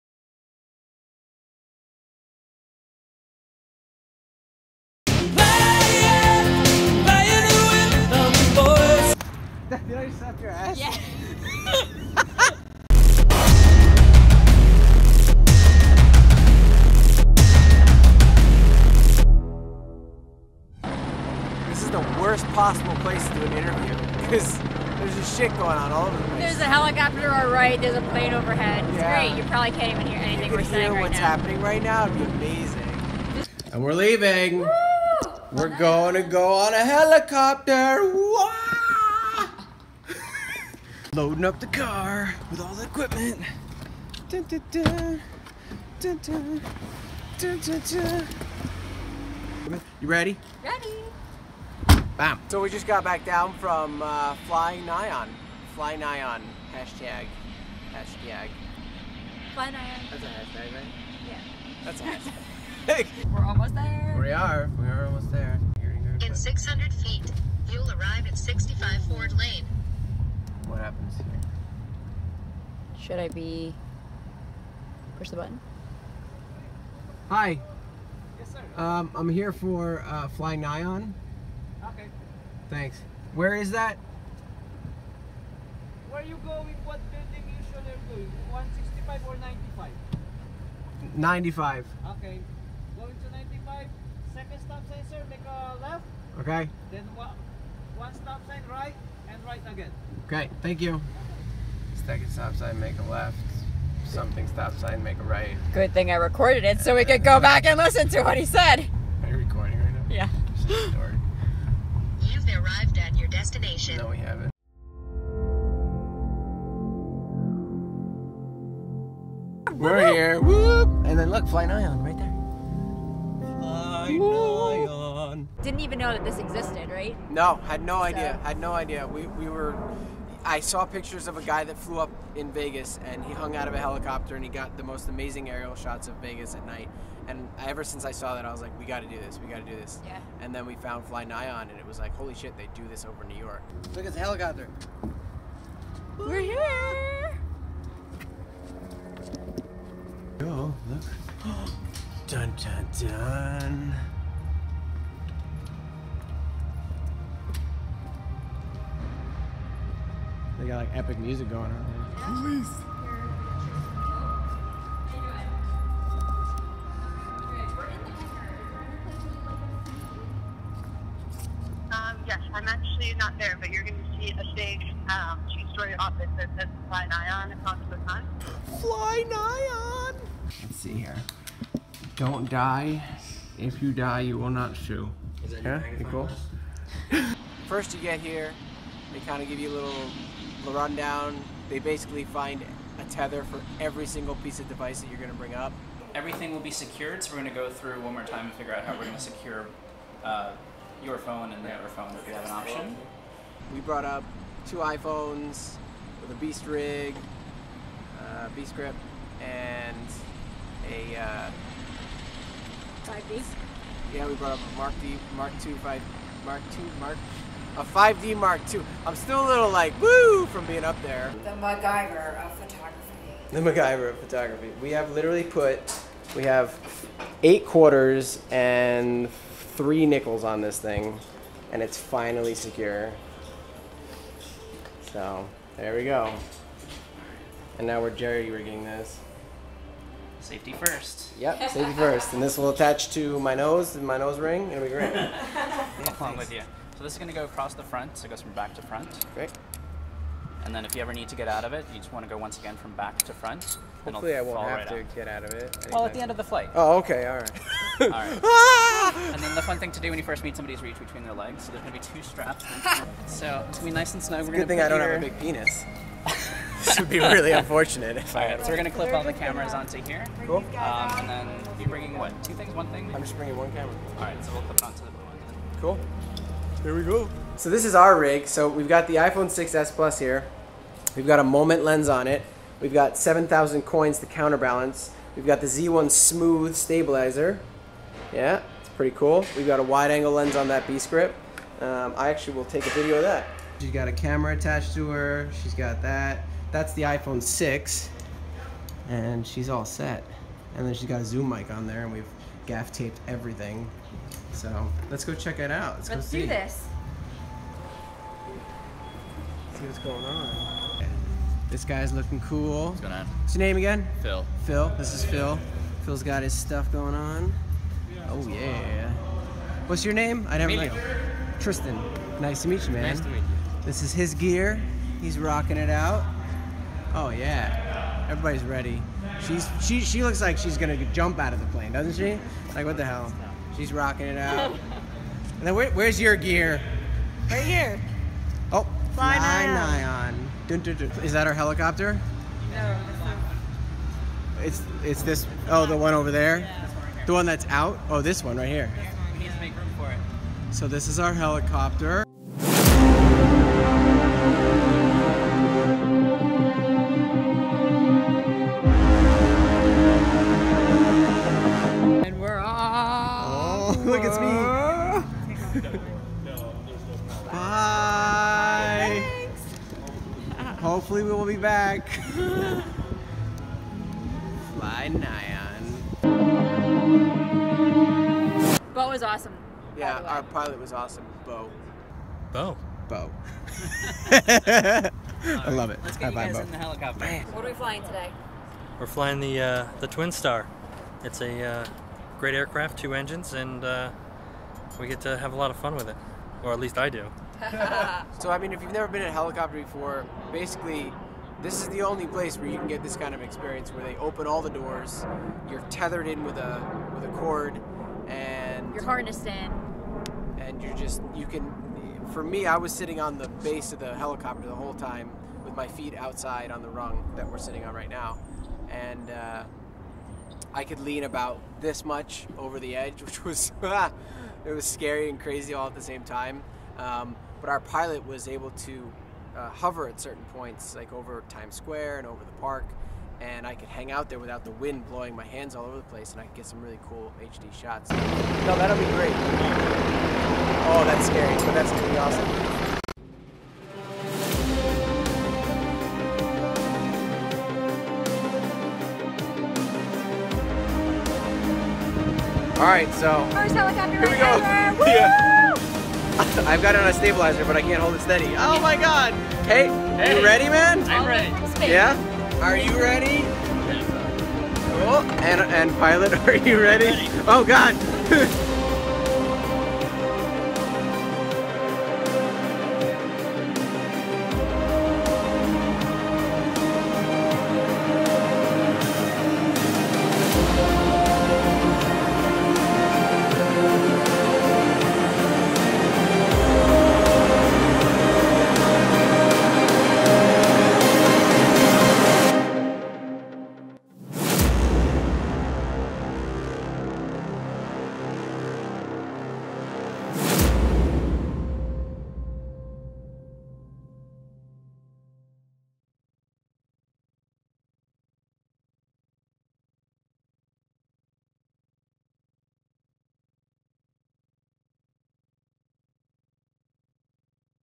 Extreme Helicopter Ride over NYC shot on iPhone in 4K! "Part 1"
Tristan Pope Photography took to the skies over NYC with the iPhone 6S + to get some of the first 4K aerial footage via an amazing company called FlyNyon. They fly open door helicopters directly over some of the most amazing sights NYC has to offer, deemed as "the Ultimate Photo Experience". And it truly is! See what it is like to FlyNyon and how our different gear holds up against the extreme conditions! Will the screen break? lol

All aerial shots are in Native 4k on the iPhone 6S+
All Behind the Scenes are in 1080p on the iPhone 6+
Footage rendered at 4K

Equipment used:
Moment Lens
Blackwing handle
Reticam Mount
Z1-Smooth Stabilizer
Zoom iQ5 Lightning Adapter Mic
Shit tons of Gaff tape
and a lot of tethers!